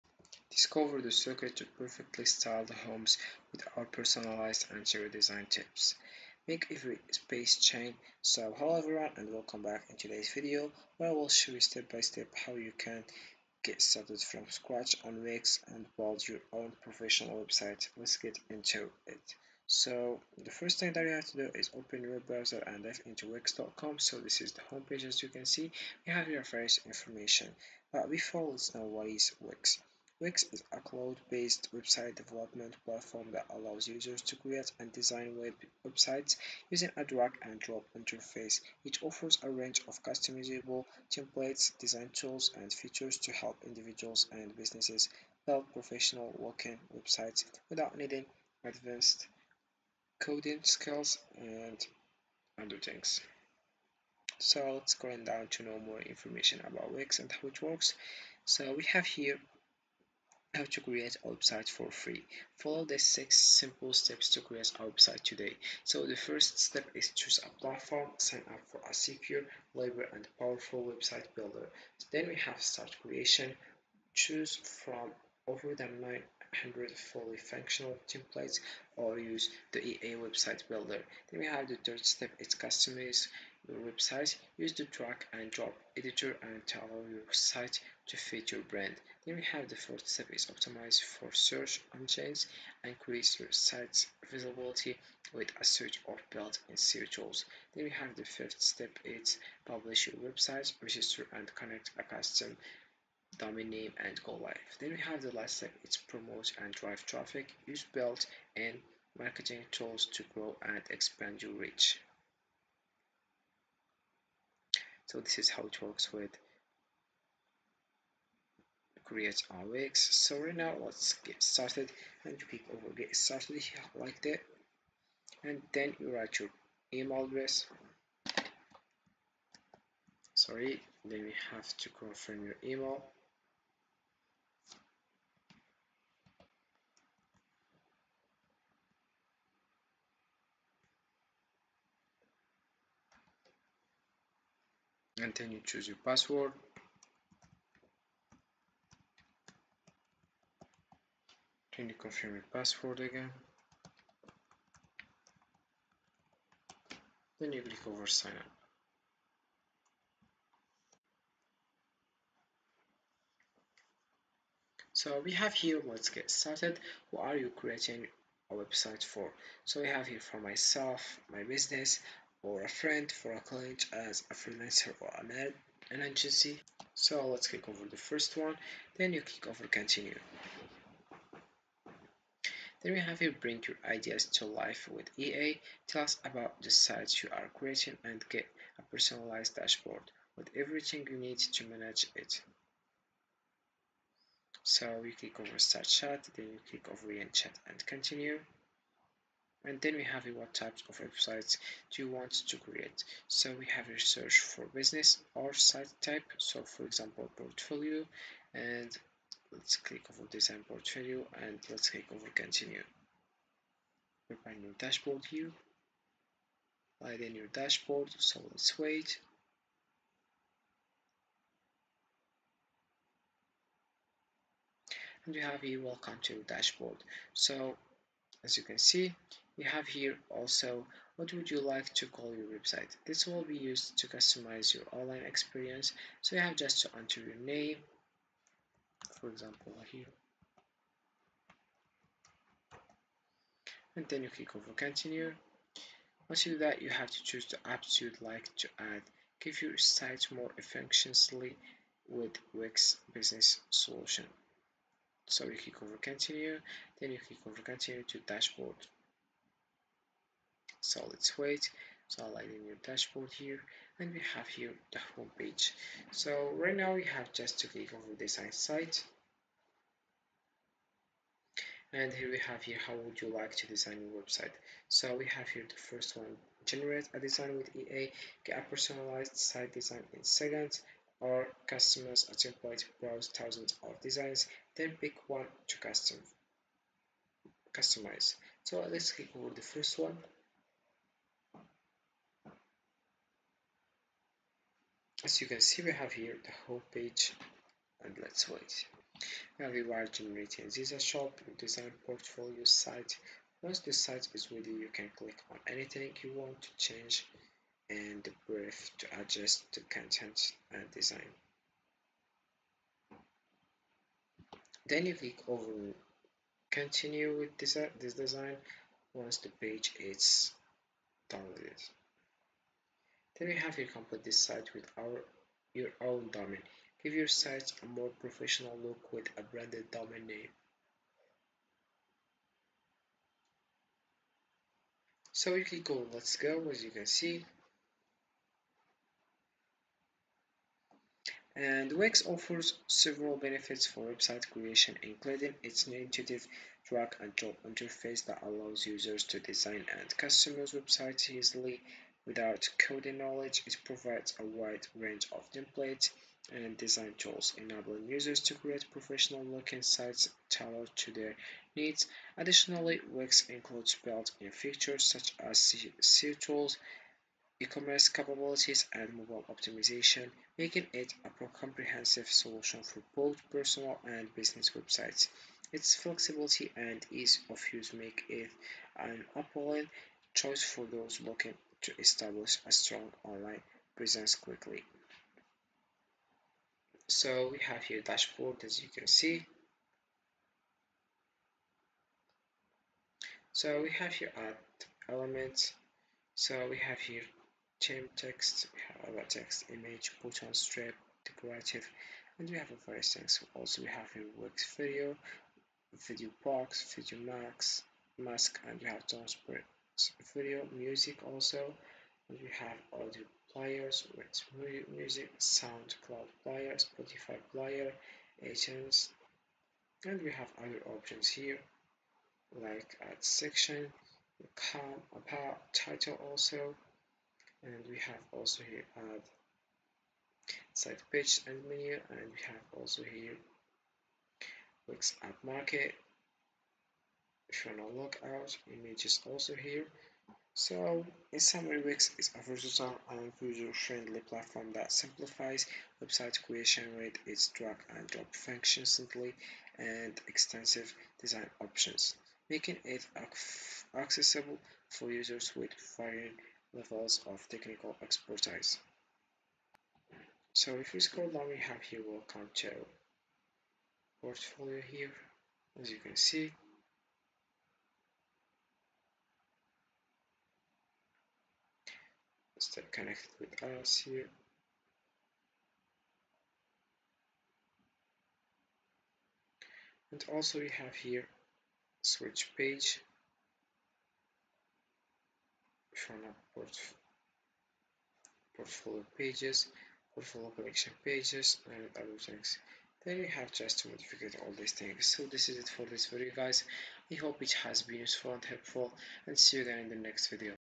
How to Create a Wix Website in 2025 : Everything You Need to Succeed
How to Create a Wix Website in 2024: Everything You Need to Succeed

in this video we discuss :